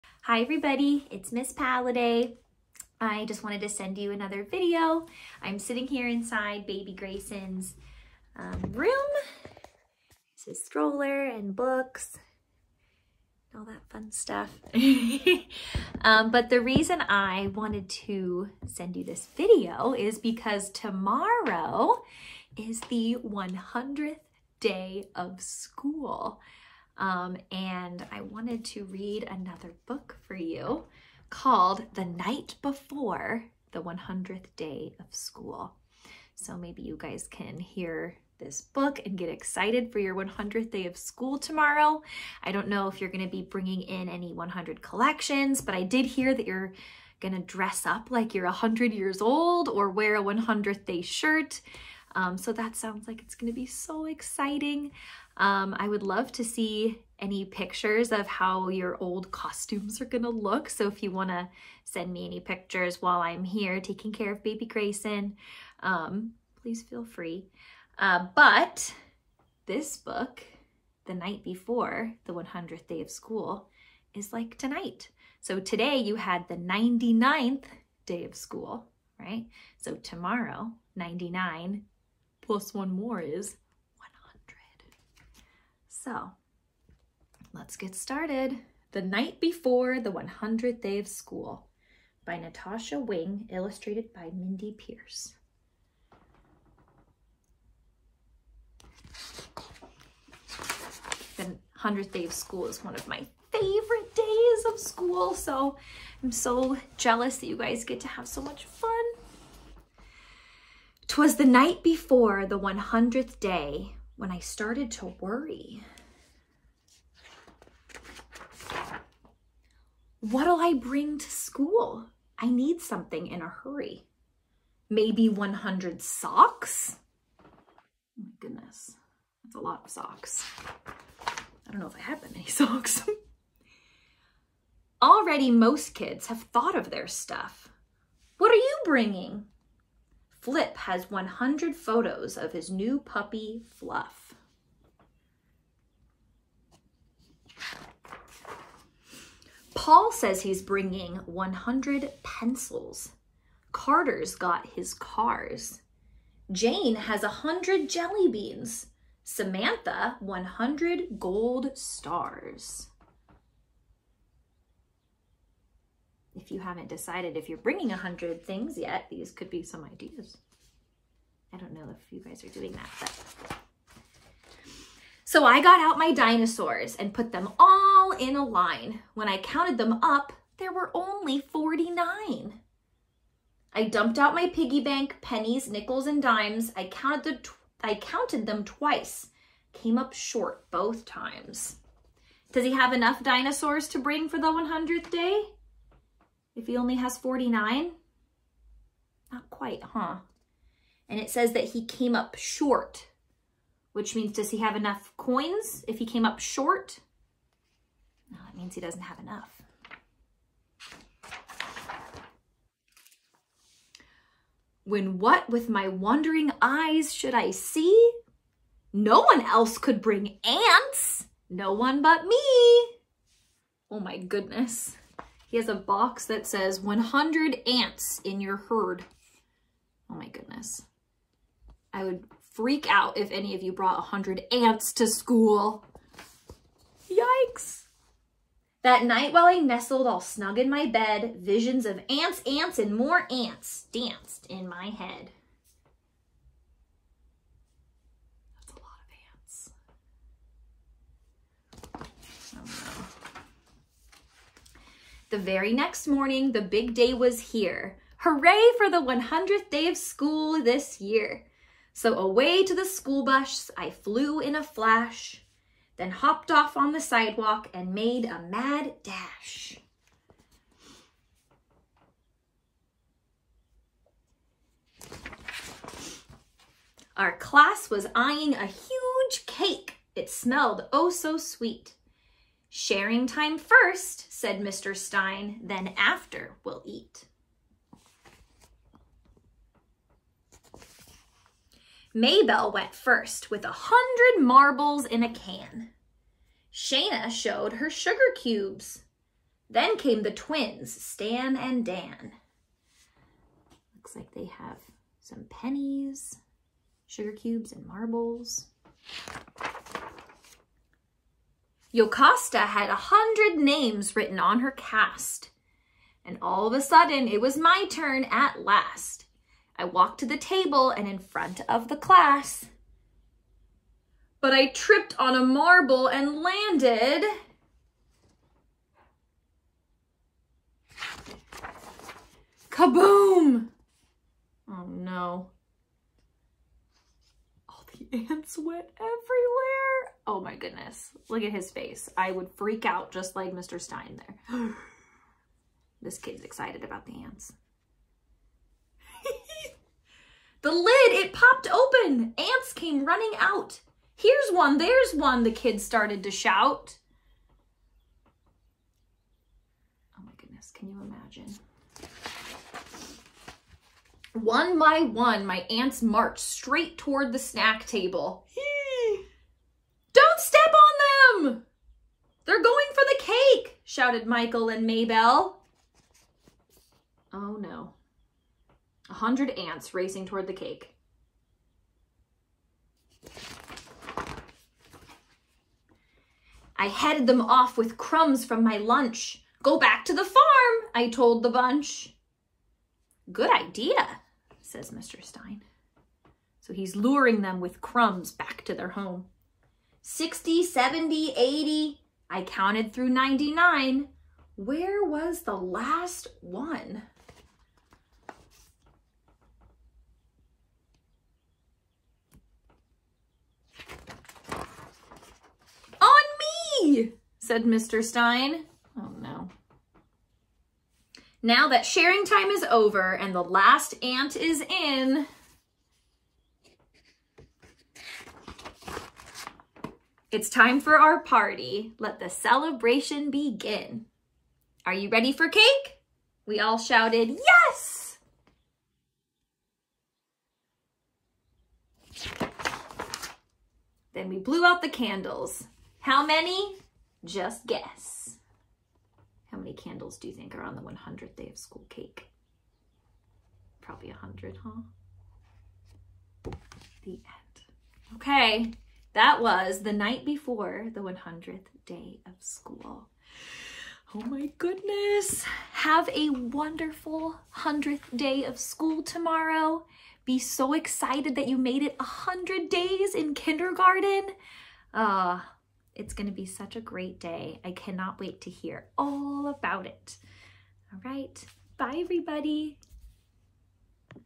The Night Before the 100th Day of School
Hi everyone! I am so sad that I will be missing out on the 100th day of school this year, but I still wanted to participate and read to you one of my favorite books, The Night before the 100th day of School! :) I hope you have an awesome day tomorrow!